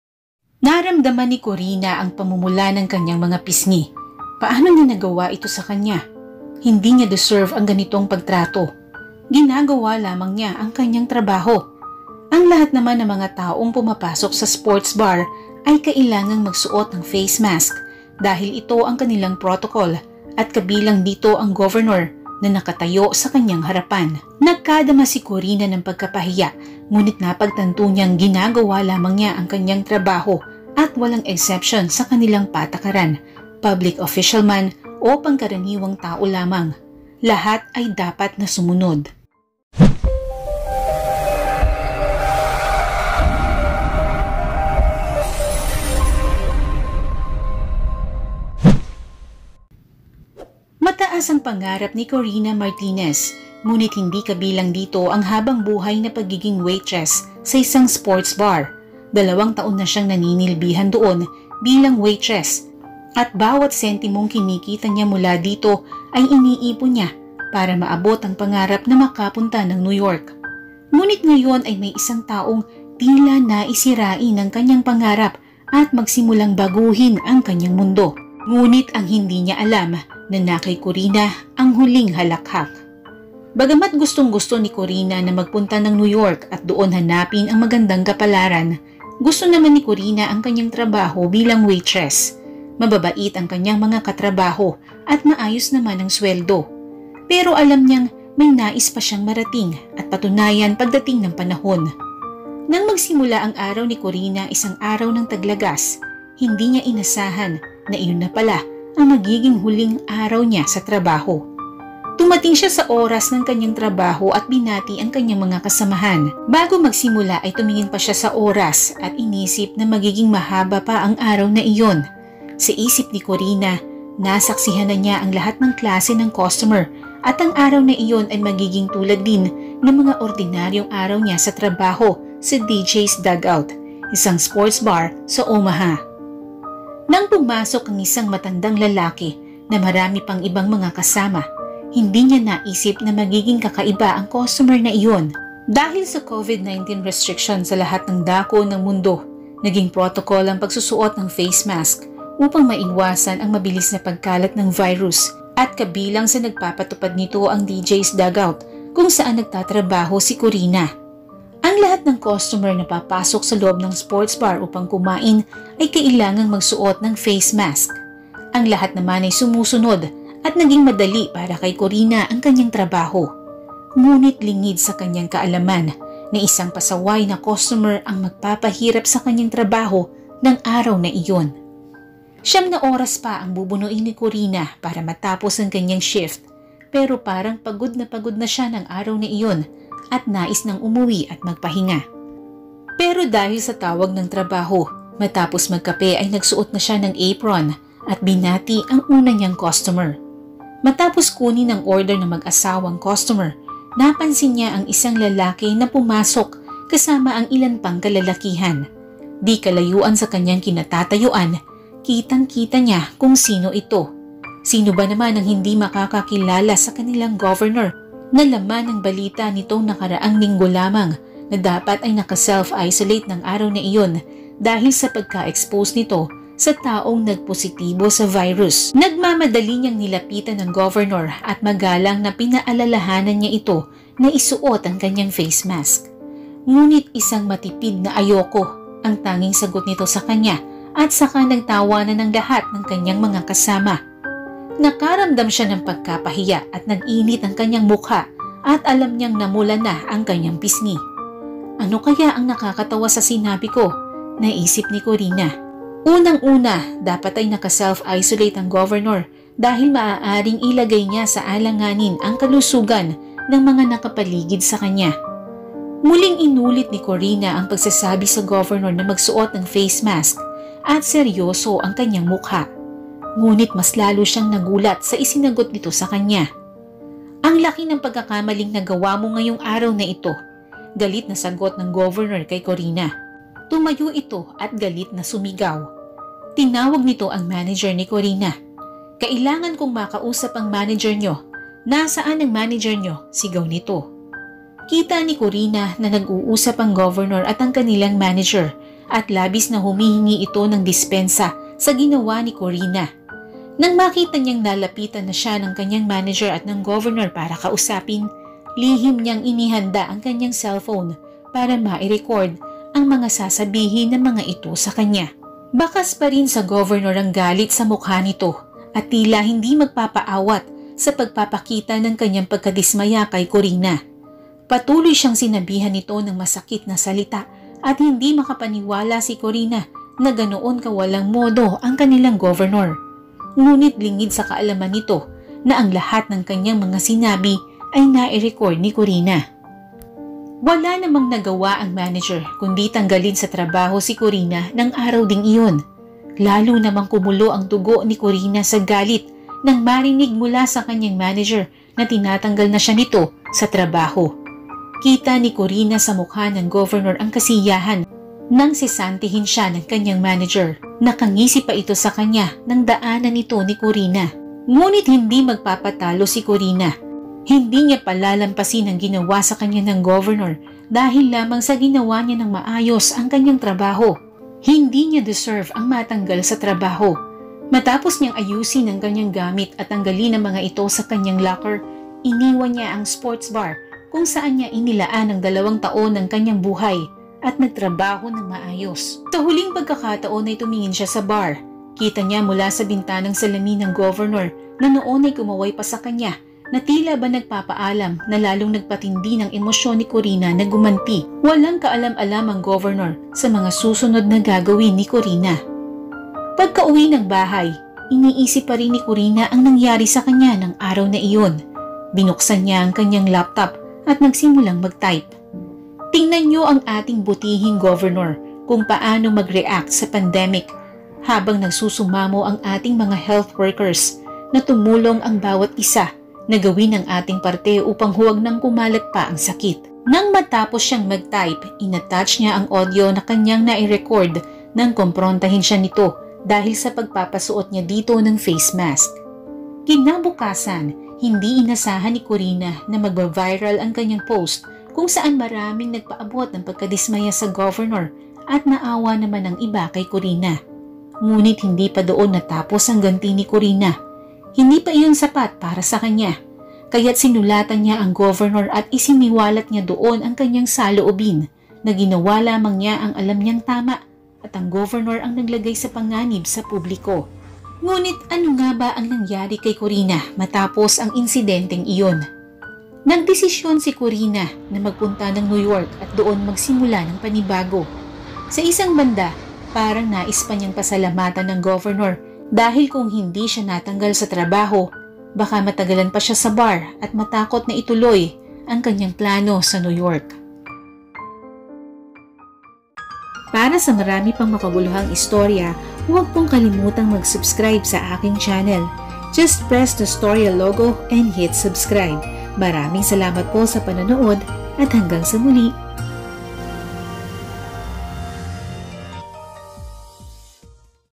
Nasibak siya sa trabaho dahil sa pagpapasuot ng face mask sa governor at ito ang kanyang iginanti!
Naramdaman ni Korina ang pamumula ng kanyang pisngi. Paano niya nagawa ito sa kanya? Hindi niya deserve ang ganitong pagtrato. Ginagawa lamang niya ang kanyang trabaho. Ang lahat naman ng pumapasok sa sports bar ay kailangang magsuot ng face mask dahil ito ang kanilang protocol at kabilang dito ang governor na nakatayo sa kanyang harapan.